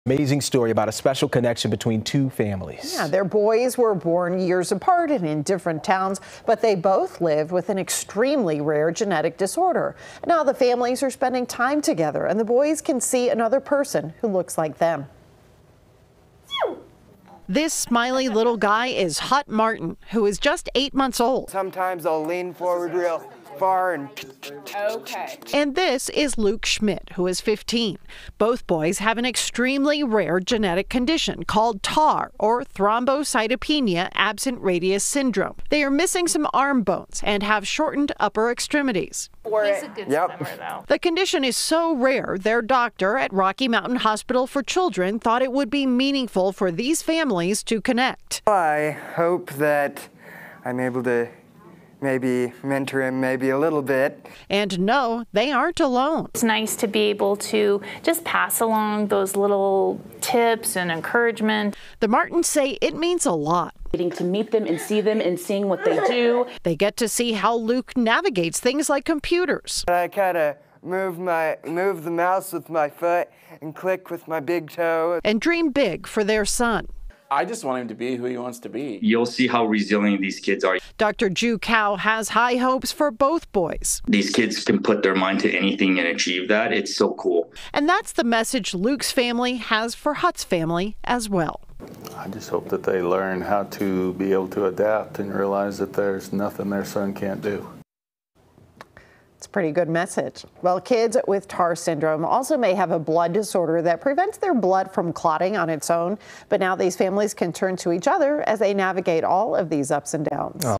Boys with the same rare genetic disorder get to meet
Hutt Martin is just 8 months old, and Luke Schmidt is 15. Both boys have an extremely rare genetic condition called TAR, or Thrombocytopenia Absent Radius Syndrome, which among other things can cause a shortening of the upper extremities and a blood clotting disorder.